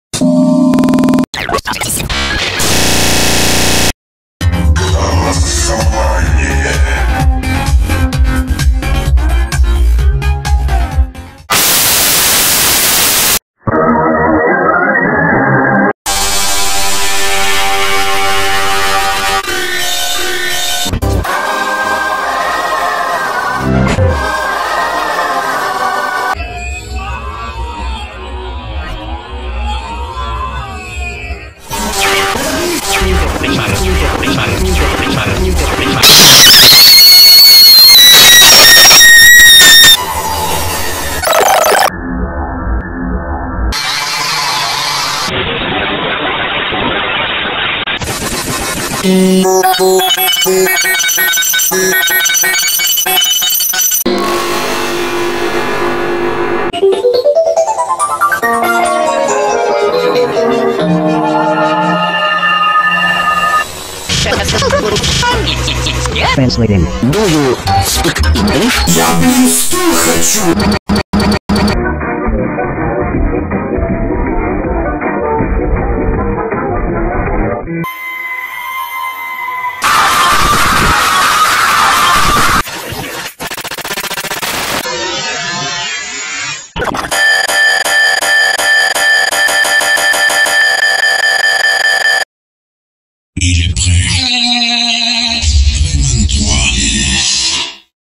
[TENNIS] And recently, Rayman just have bought RaymanPad 5.30
Round 37/58 of TRVT vs myself. First video using VideoPad 5.30